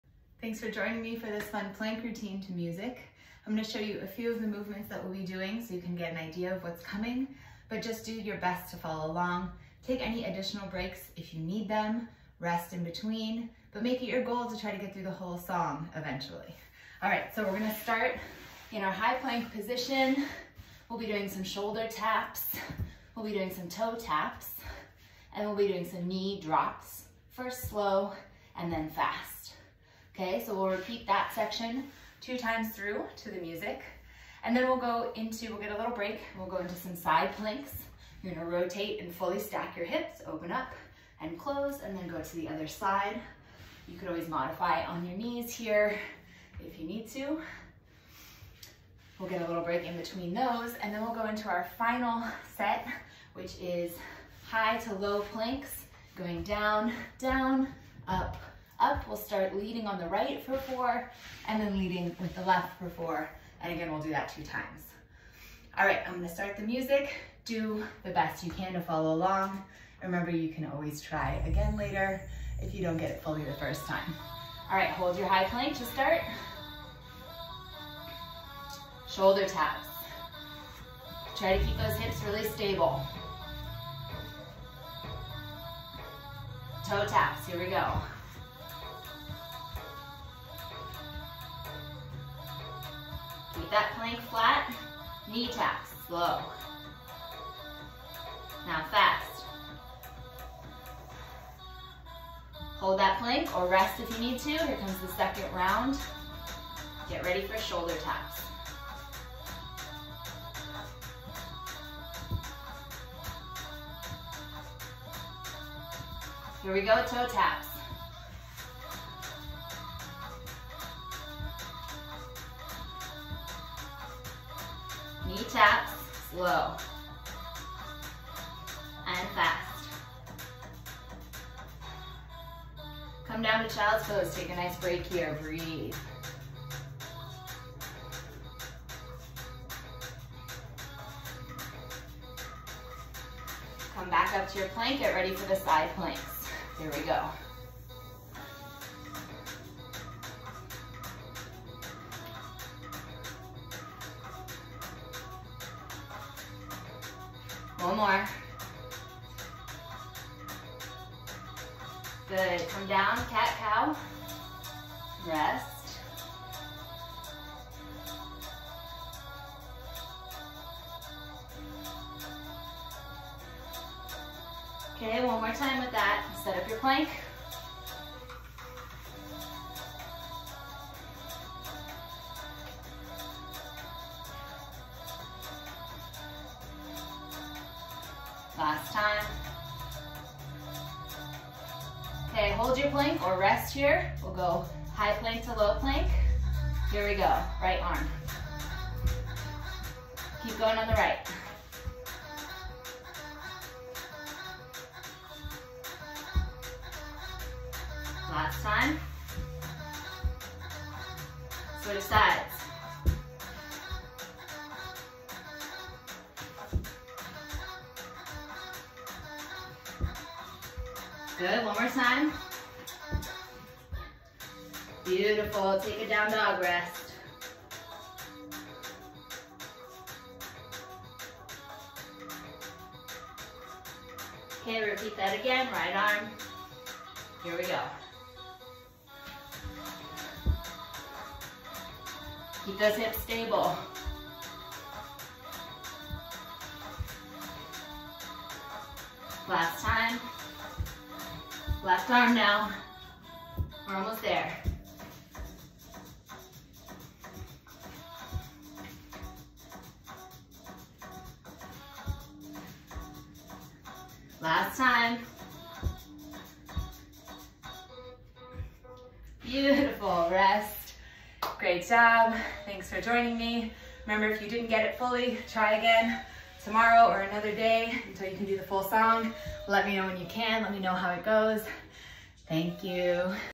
Plank Challenge to Music
Try out this fun plank challenge!  Keep trying until you can get through the whole song without extra breaks.

Want another core challenge?